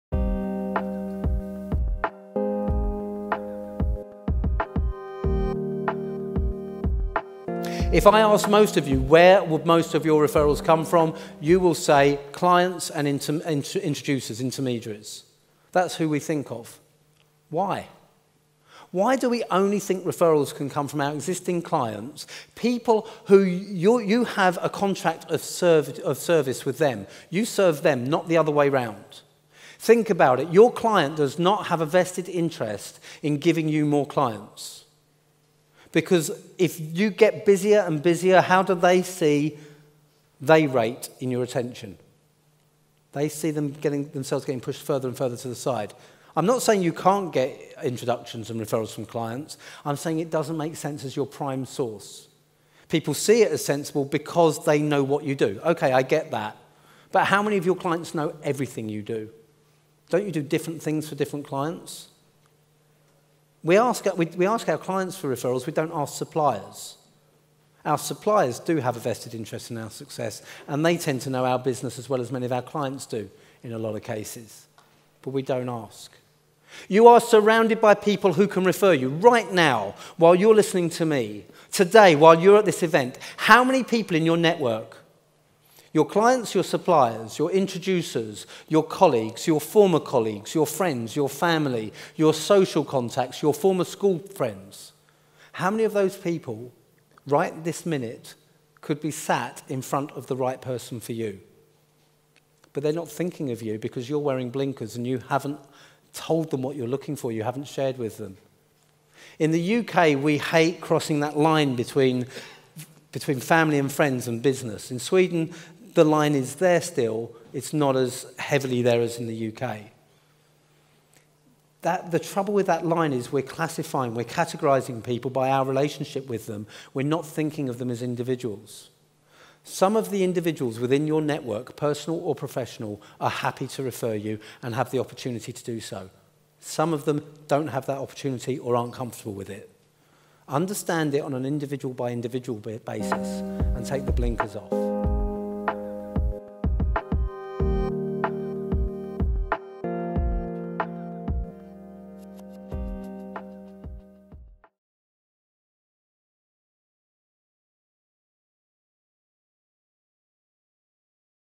Take the Blinkers Off! Who Aren't You Asking for Referrals?
Business Networking Strategist Andy Lopata wants to challenge you to look at who you are turning to for referrals. You're missing out on many opportunities!